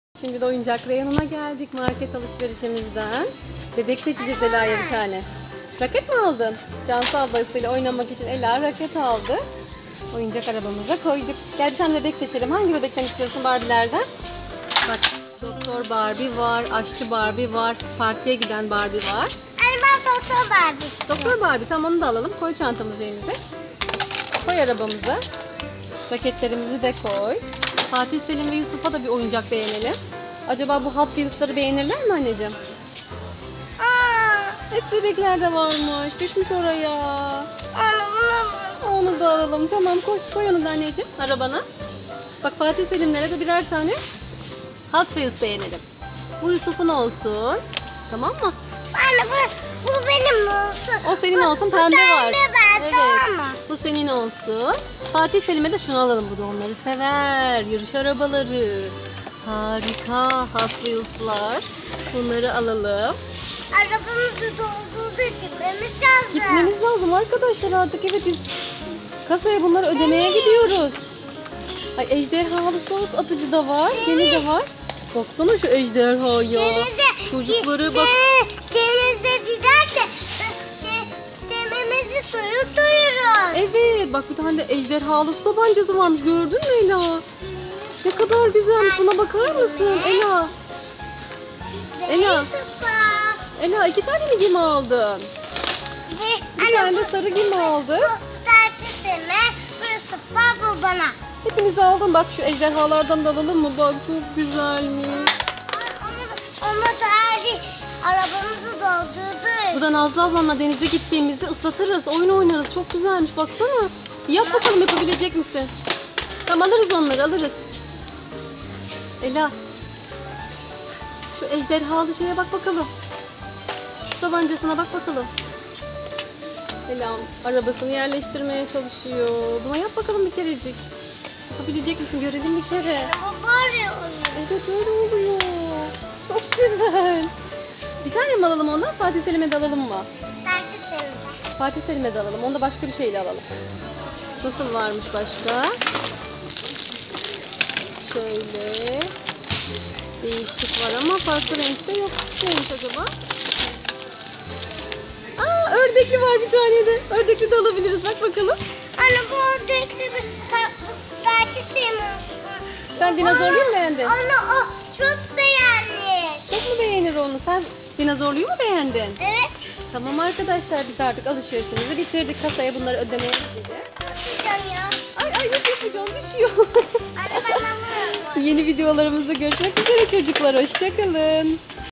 Oyuncak market arabasıyla markete gittik, Fatih selim ve Yusuf a hot wheels oyuncak araba aldık🚗
Bu gün oyuncak market arabasıyla markete gittik, Fatih selim ve Yusuf a hot whells oyuncak araba aldık.Ela diğer oyuncakları da inceledi.Barbie bebekler, tenis raketi, ejderhalı su tabancası, gemiler ve birçok oyuncakları inceledi.Bir araba oyuncak aldı.Çok mutlu oldu.Videomuzu beğenmeyi ve abone olmayı unutmayın arkadaşlar.Hepinize sevgilerimizi gönderiyoruz.